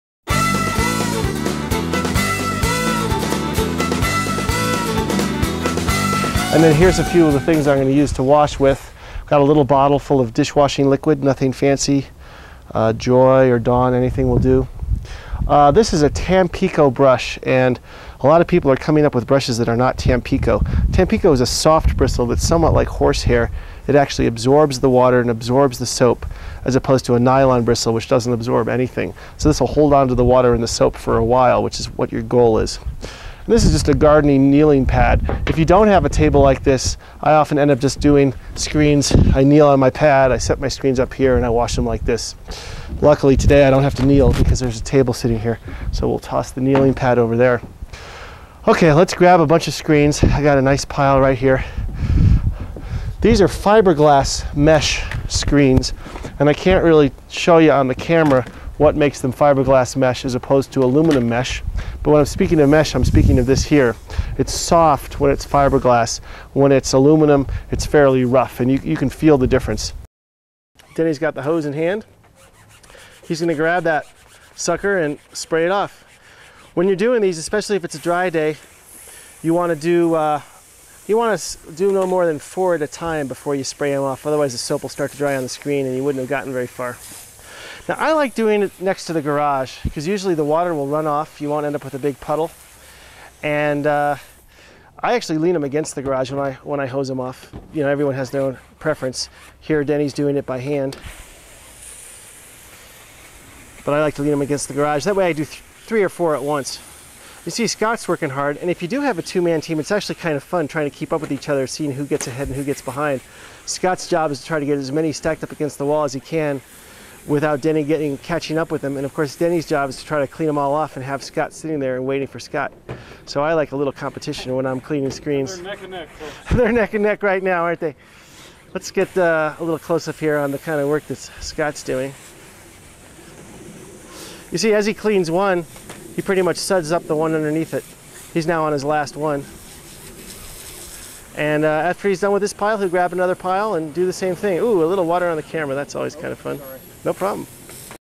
How-to wash screens with Dr.Glass
A short screen washing tutorial from the Dr. Glass window washing team.